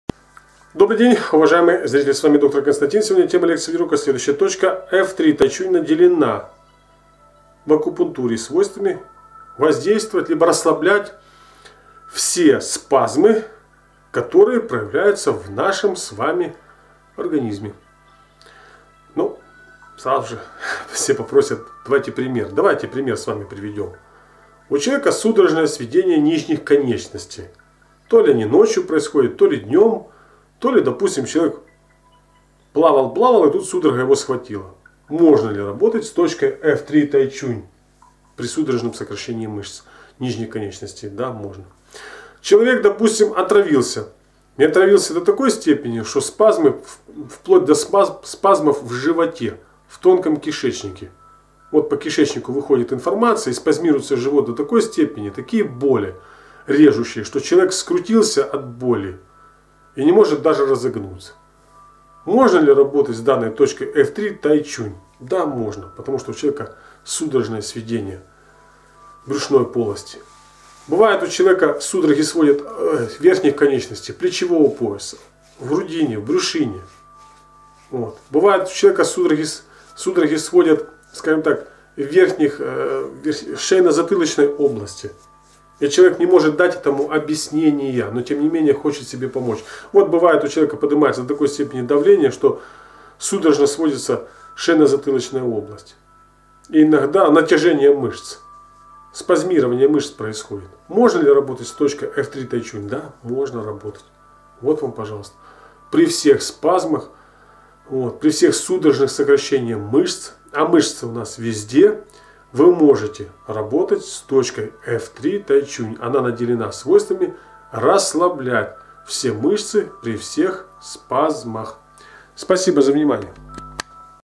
Помогаем при всех СПАЗМАХ!
Помощь акупунктурными точками при всех спазмах у человека. Видео-Лекция Предоставлена Доктором Константином.